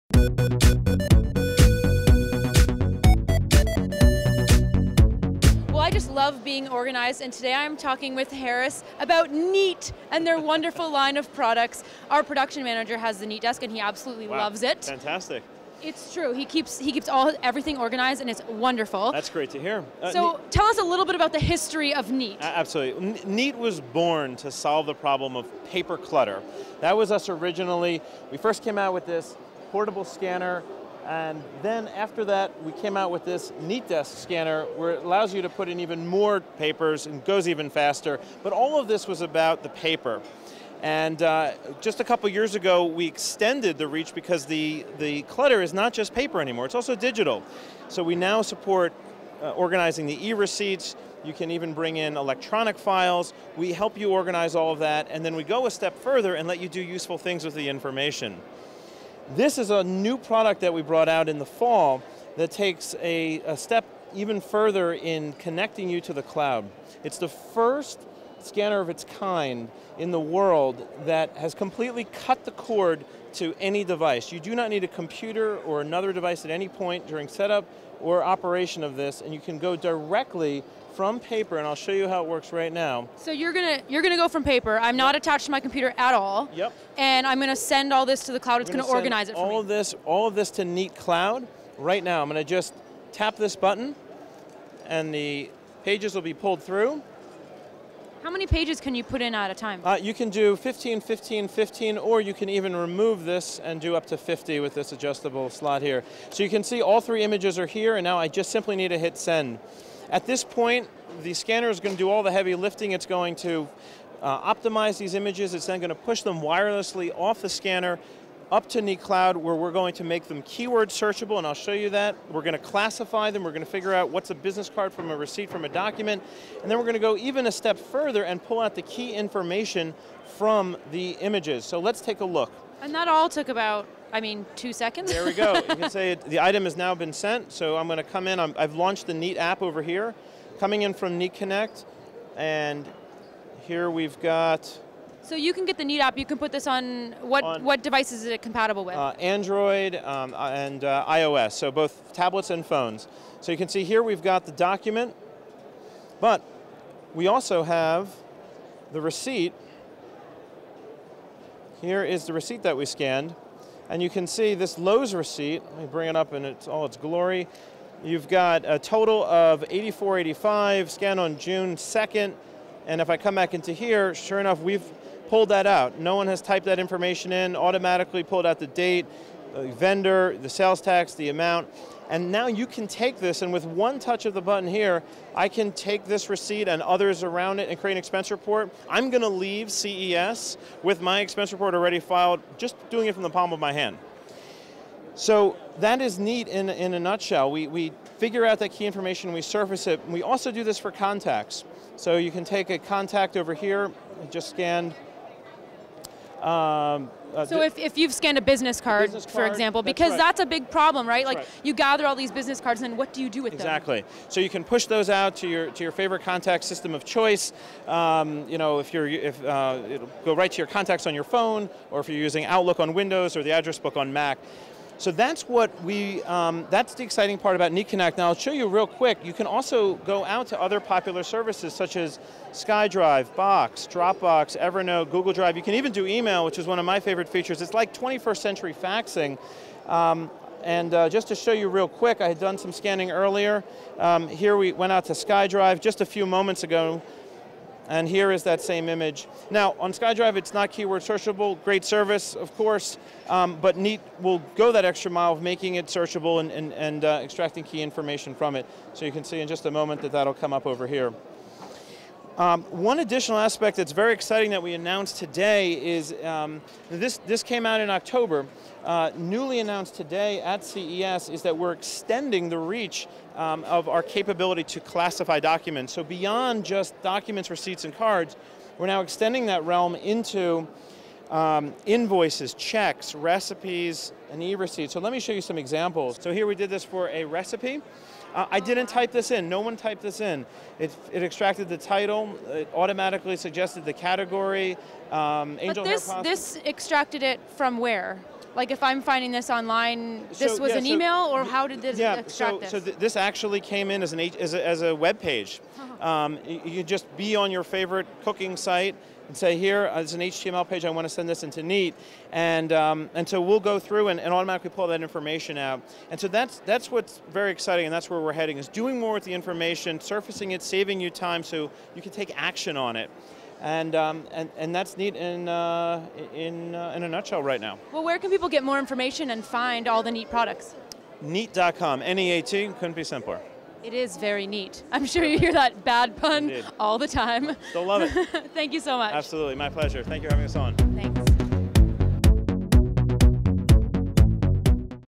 NeatConnect at CES 2014
Host Tasia Custode is at the Neat booth at ShowStoppers at CES 2014, checking out the new NeatConnect cloud scanner and digital filing system. Harris Romanoff gives her the scoop.